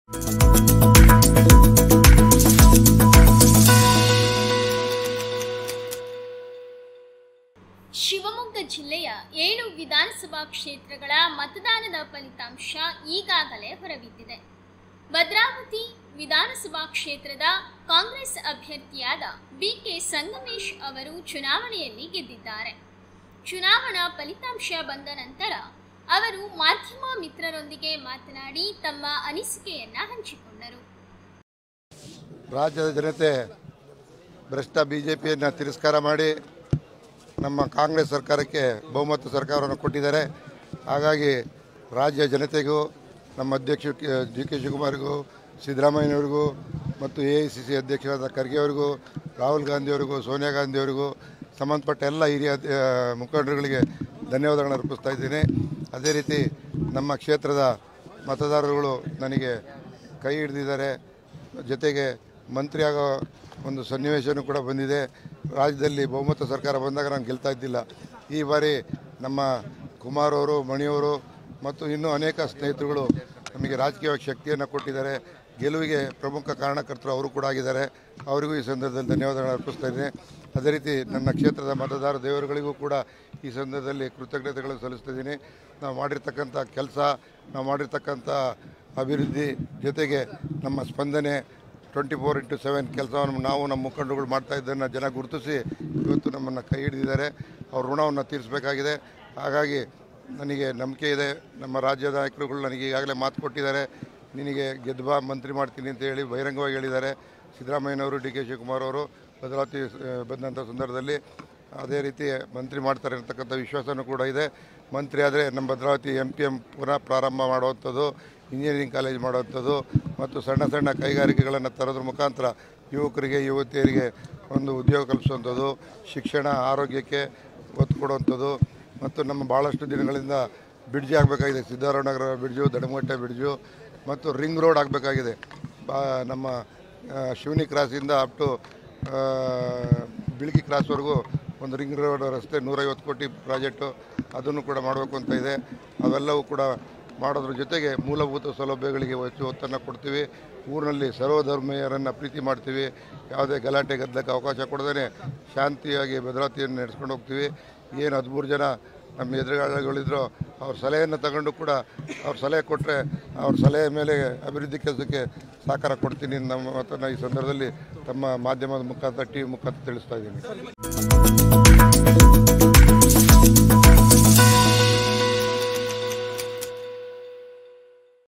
B K Sangamesh | Kannada News
B K Sangamesh | ಭದ್ರಾವತಿ ವಿಧಾನಸಭಾ ಕ್ಷೇತ್ರದ ನೂತನ ಶಾಸಕರಾಗಿ ಆಯ್ಕೆಯಾಗಿರುವ  ಬಿ.ಕೆ. ಸಂಗಮೇಶ್ವರ